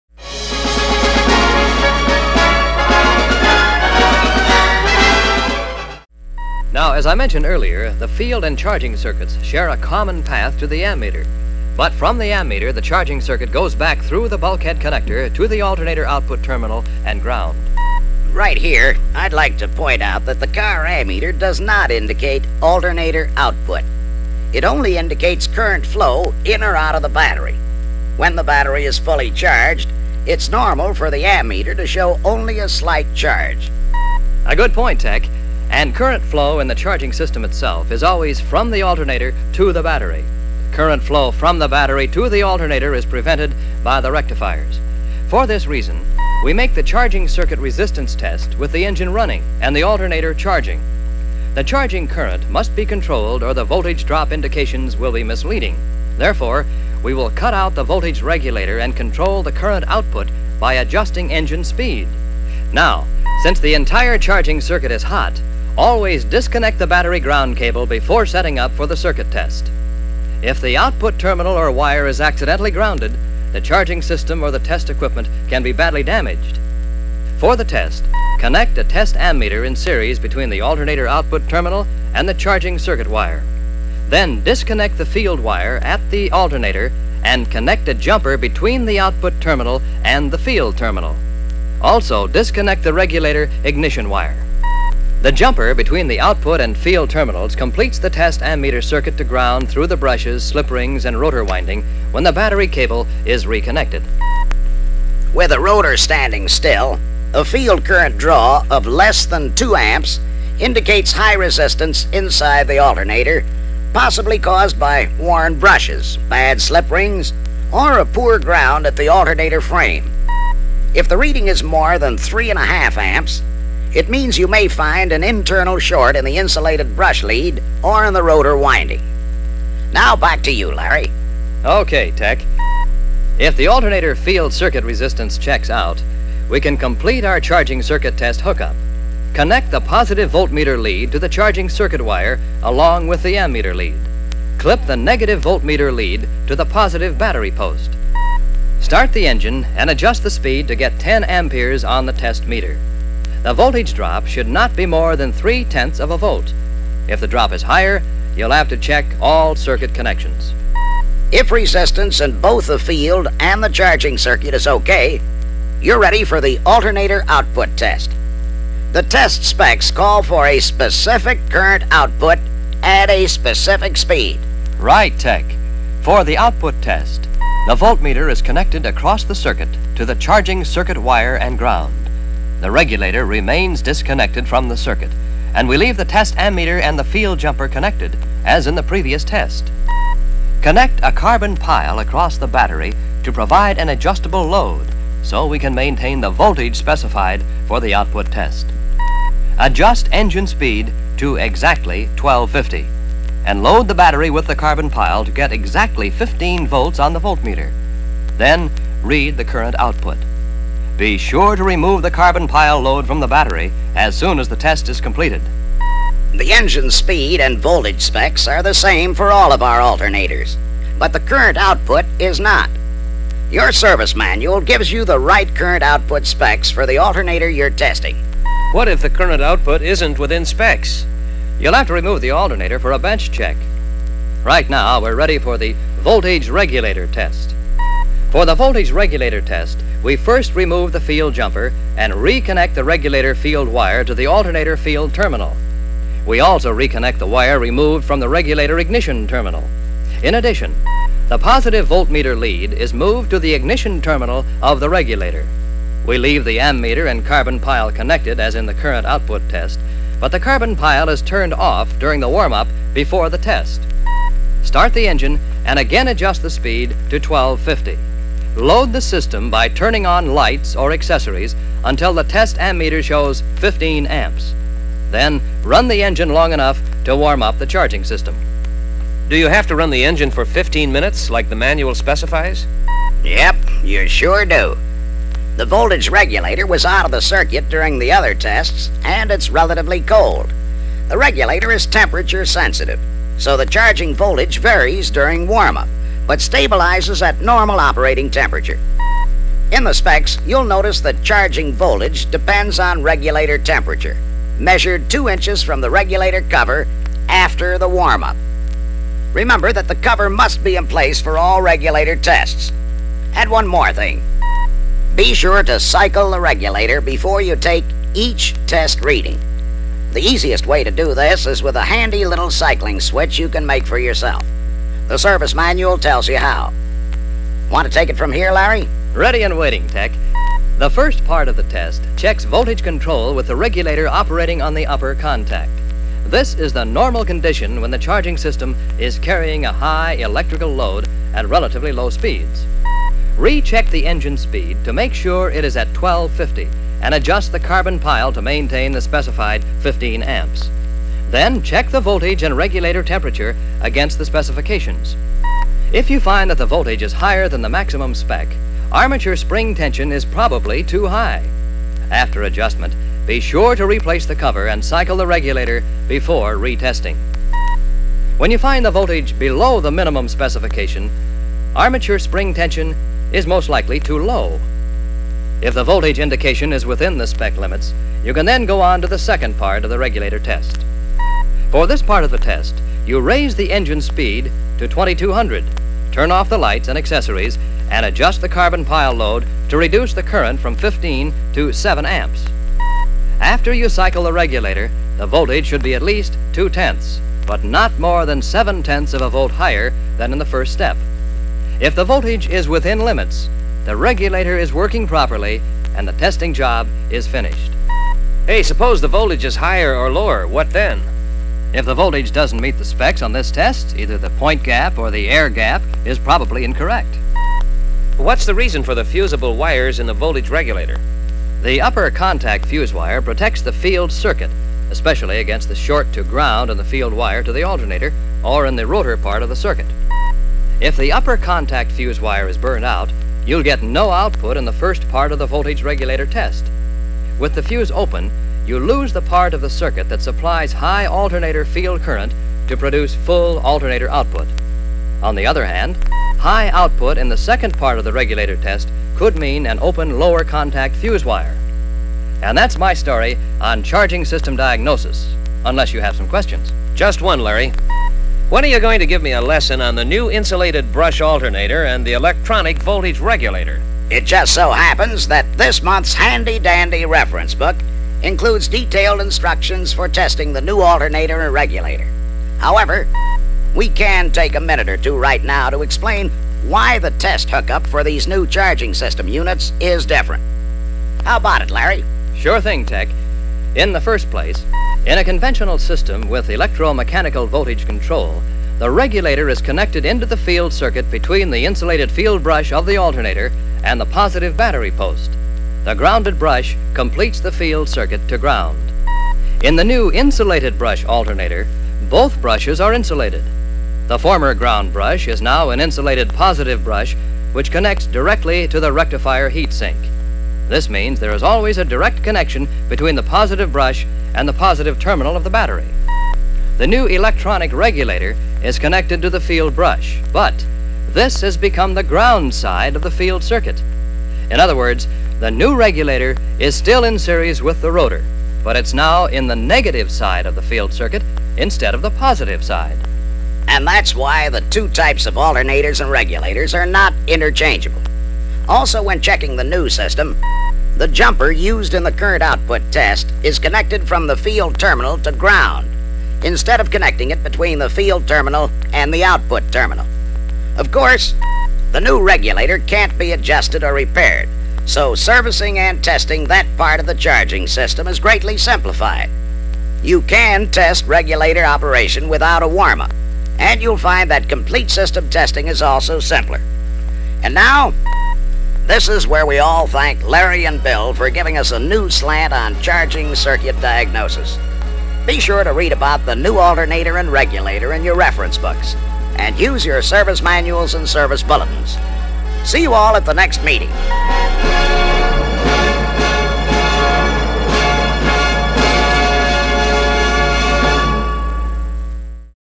69 3 Charging System Pt 2
Mechanic training on the 1969 Mopar charging system. Part two of two. Find more vintage Mopar material at Hamtramck-Historical.com